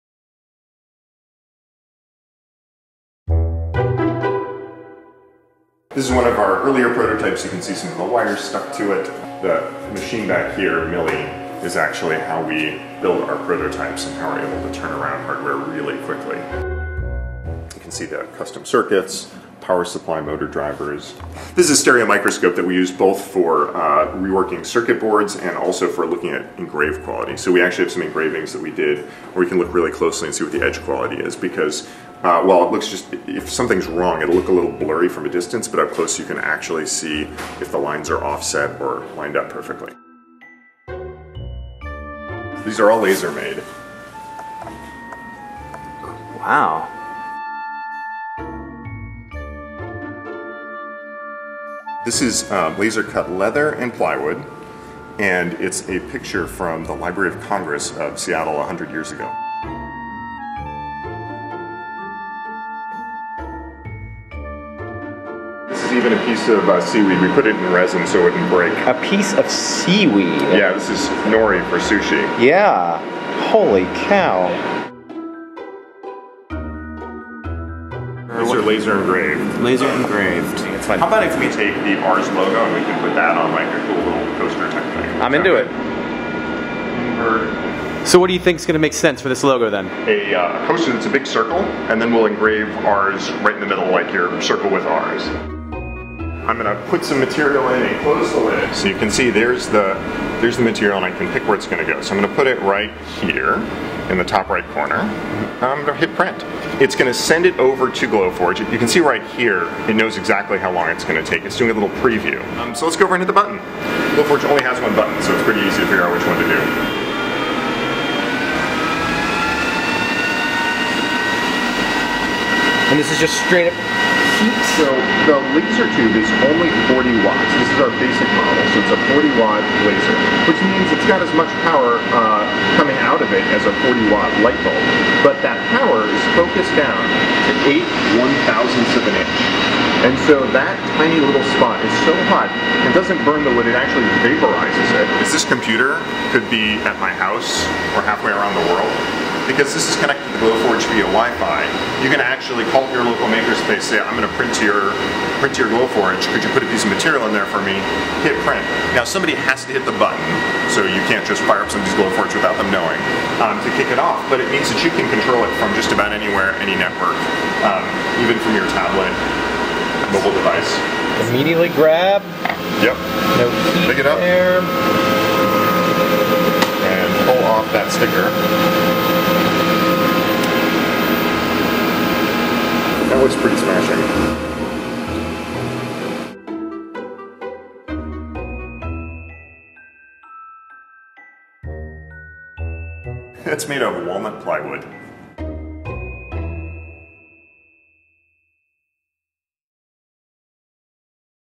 Ars Visits Glowforge 3D Printing
Ars' Sam Machkovech visits Seattle-based Glowforge and prints an Ars coaster with a 40-watt laser.